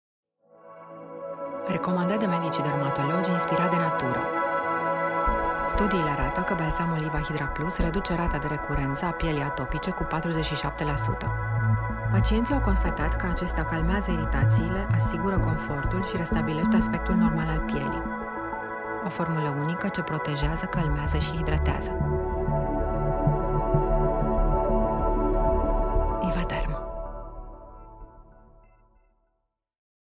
Ivahidra+ Balsam intens relipidant este recomandat de către dermatologi - Ivatherm
Inovația Ivahidra+ Baume este ideală pentru îngrijirea pe termen lung a pielii sensibile a bebelușilor, copiilor și adulților. Noua formulă oprește mâncărimea, calmează iritația și hidratează pielea în profunzime.

Emolientul Ivahidra+ Baume are o acțiune 360° asupra cercului vicios al dermatitei atopice.

Eficiența sa este dovedită clinic, părinții confirmă îmbunătățirea calității vieții după folosirea tratamentului, iar medicii dermatologi îl recomandă deoarece:

Reface microbiomul cutanat (flora pielii: microorganisme vii, prietenoase, care trăiesc pe pielea ta și lucrează pentru a o menține sănătoasă)
Hidratează în profunzime și crește integritatea pielii
Reduce inflamația și calmează pielea pe termen lung
Reface funcția de barieră a pielii
Produsul este recomandat atât pentru față cât și pentru corp, nu conține parabeni, săpunuri sau parfum.

Balsamul se aplică pe toată suprafața corpului de două ori pe zi, pe pielea curățată în prealabil cu Ivahidra+ Gel de Spălare emolient. După aplicare, pielea rămâne fină, elastică, cu aspect vizibil îmbunătățit.

Se recomandă utilizarea pe termen lung pentru distanțarea puseurilor sau în asociere cu tratamentul medicamentos.